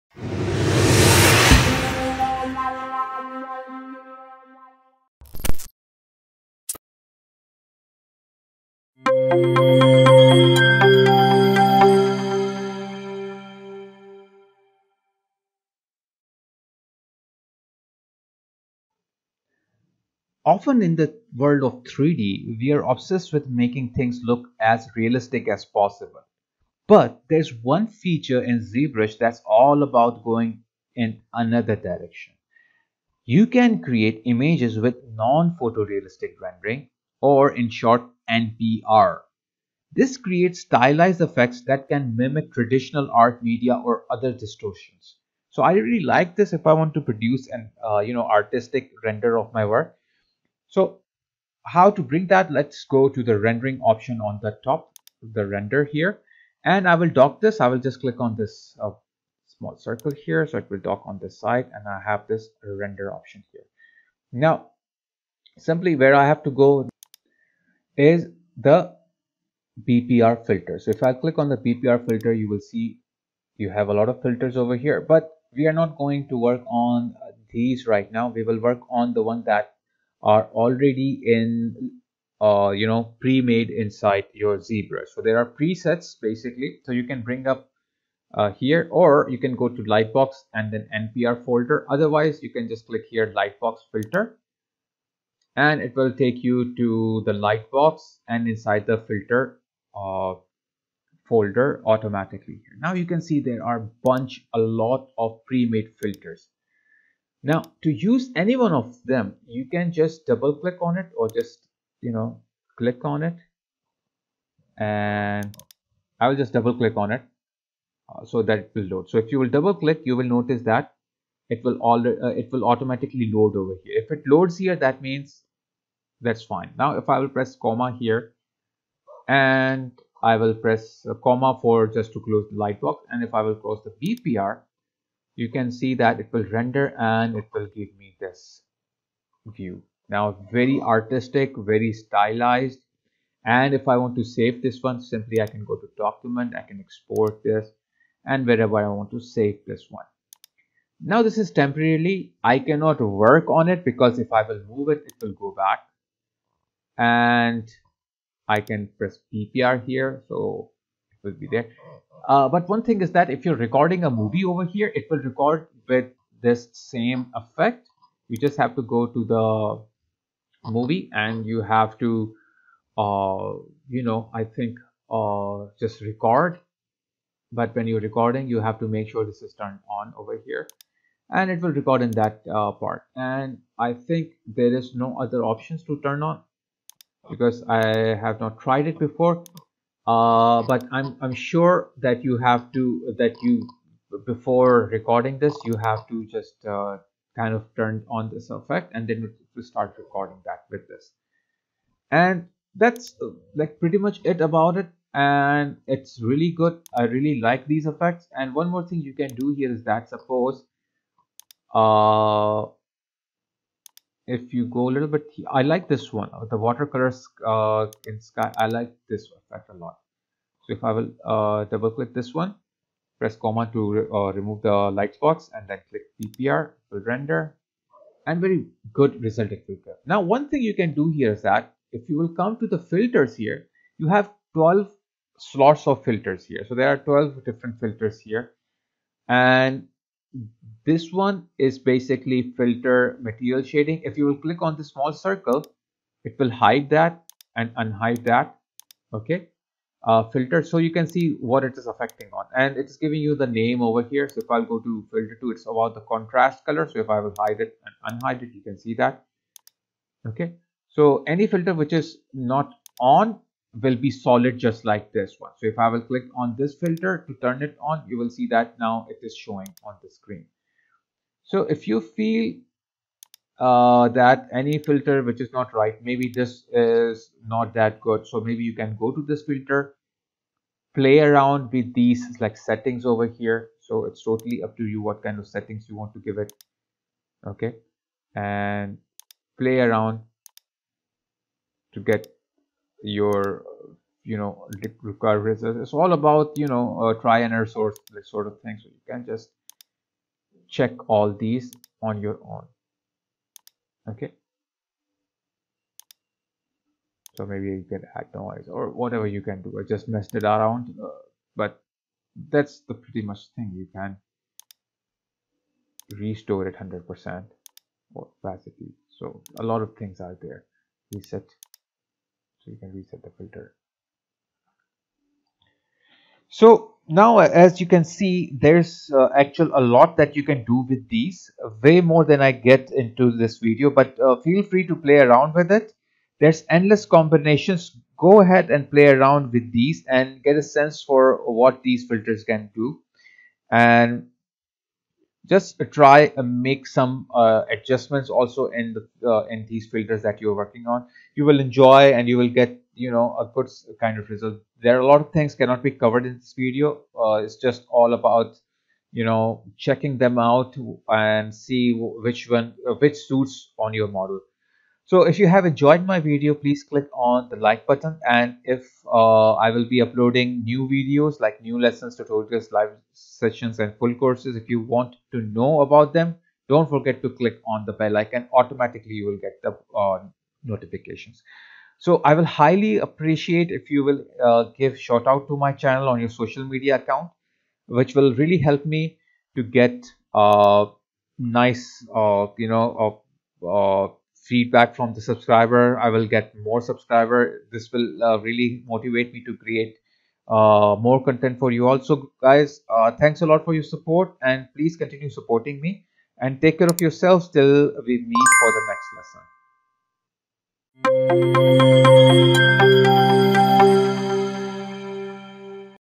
How to Render with Filters in Zbrush? | Lesson 4 | Chapter 14 | Zbrush 2021.5 Essentials Training
Often in the world of 3-D we're obsessed with making things look as realistic as possible but there's one feature in Zbrush that's all about going in the other direction. You can create images with non-photo realistic rendering, or NPR for short. This creates stylized effects that can mimic traditional art media or other distortions. Let's see how they work.